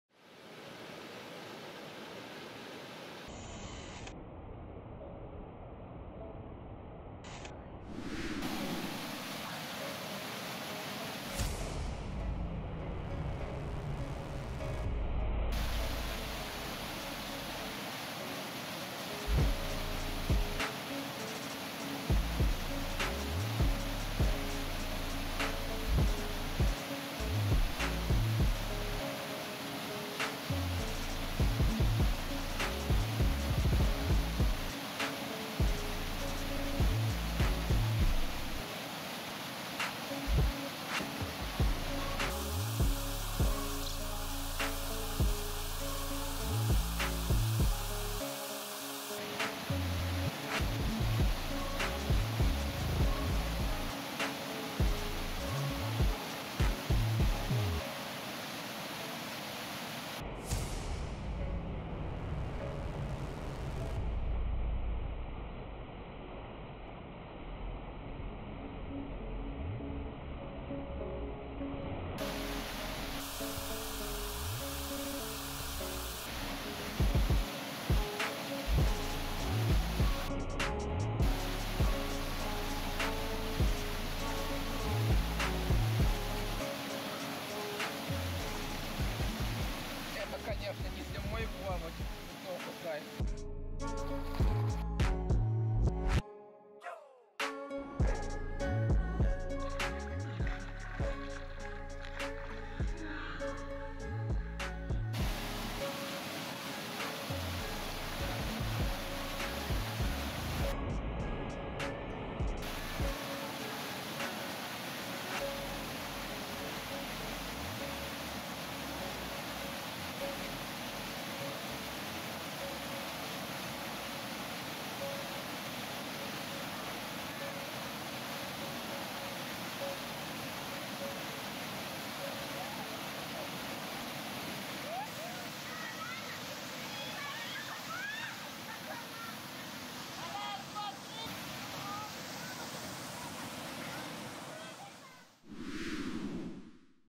Новосибирск. Беловский водопад
Новосибирская область. Беловский водопад

При съёмке не планировал ничего выкладывать, только для себя на память, но глянув исходный материал, смонтировал небольшой ролик.